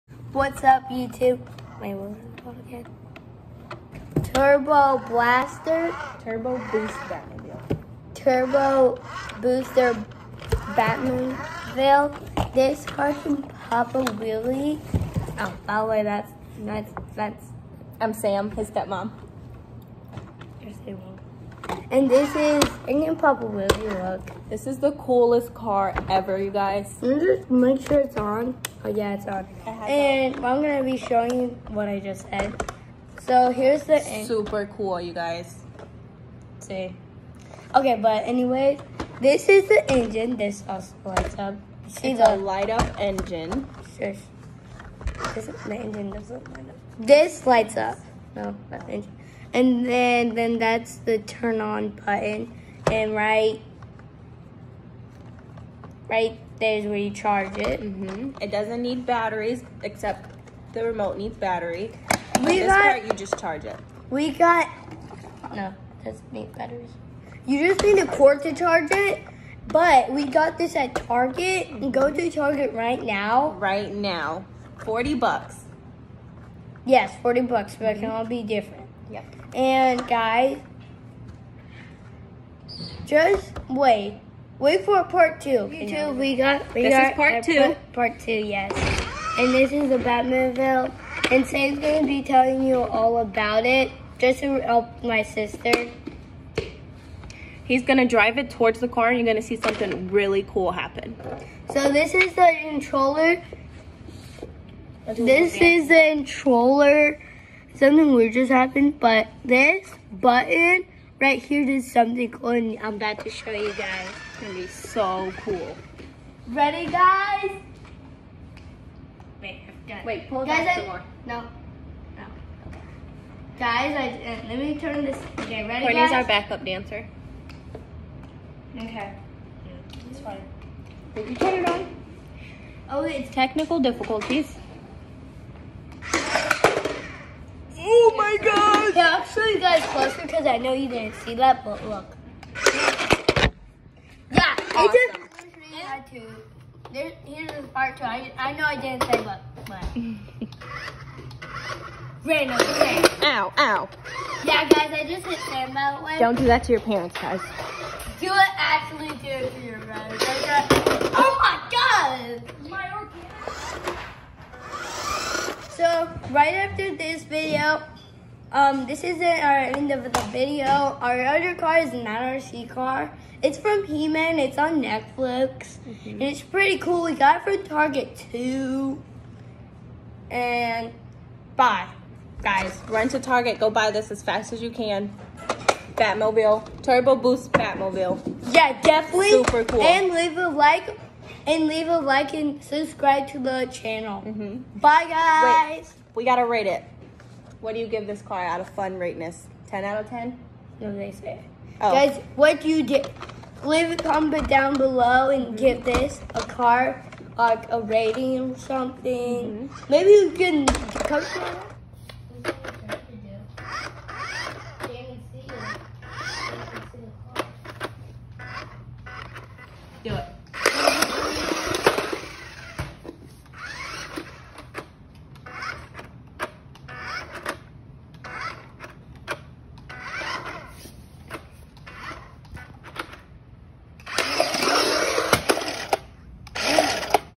Gabes review on The AWESOME Turbo Boost Batmobile.
Gabe and his stepmom reviews and plays with the new Turbo Boost Blaster! The Turbo Boost Blaster is a RC car with a light up engine and a wheelie function. It is so cool and you can get it at any Target. :)